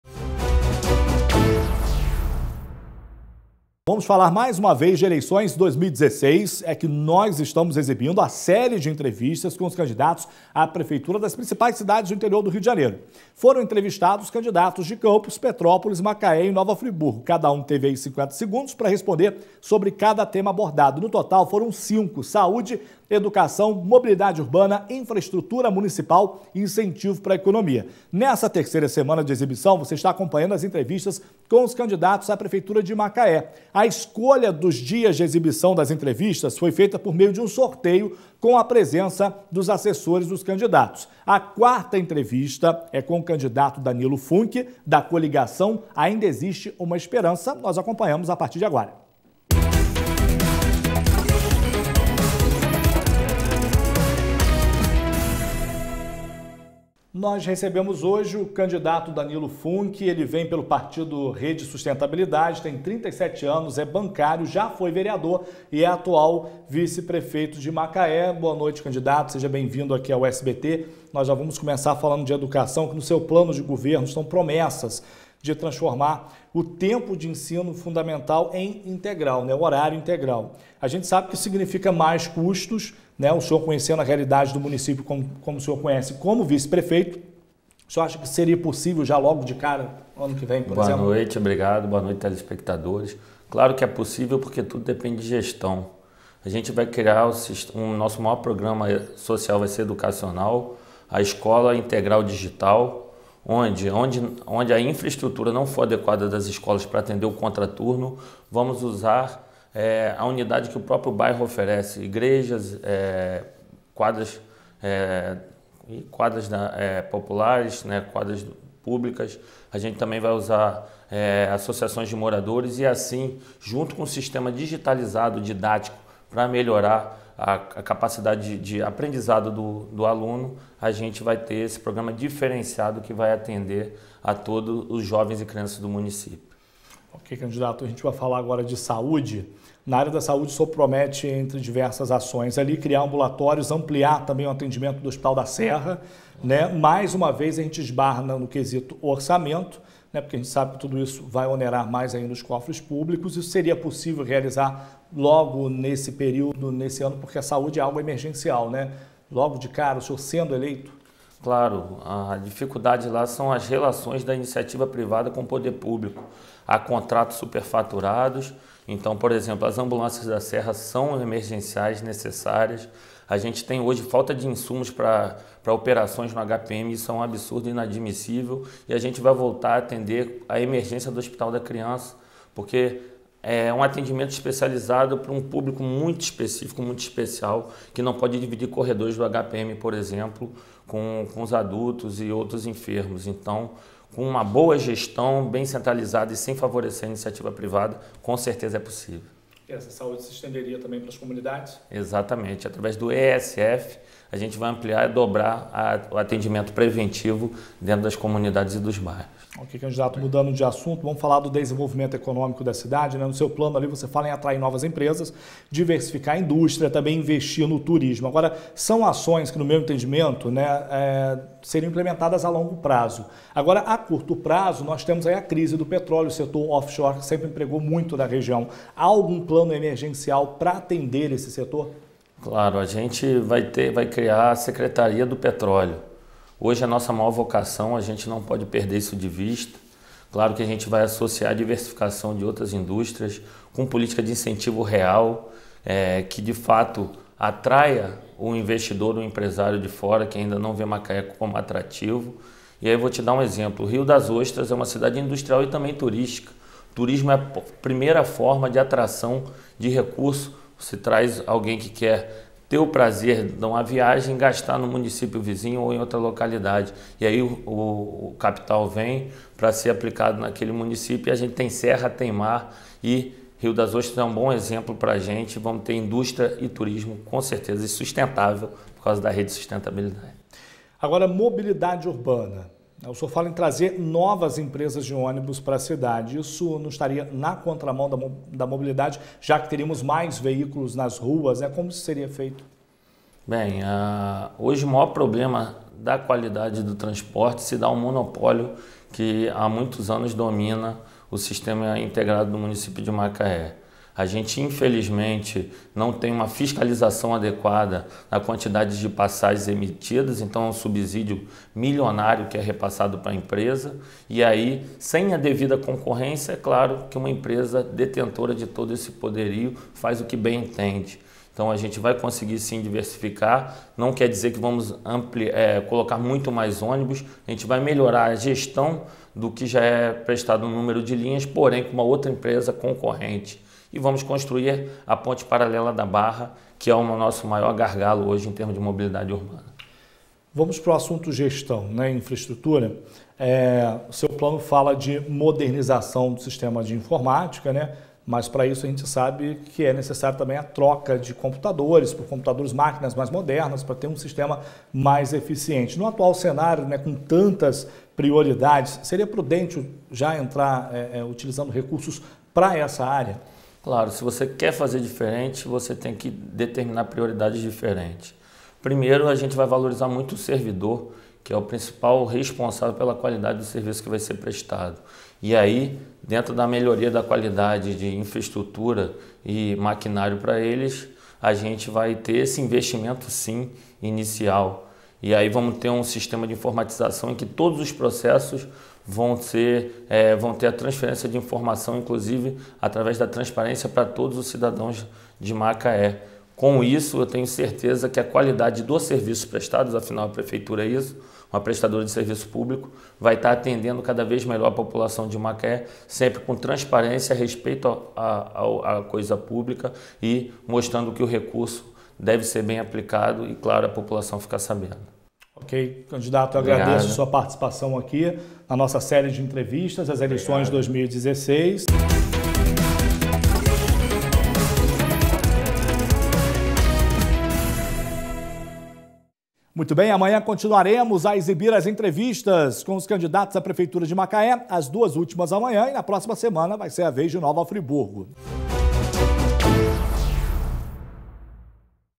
Danilo Funke, candidato à prefeitura de Macaé pela REDE, é entrevistado pelo SBT
O candidato falou sobre cinco temas pelo projeto Sabatina SBT.&nbsp;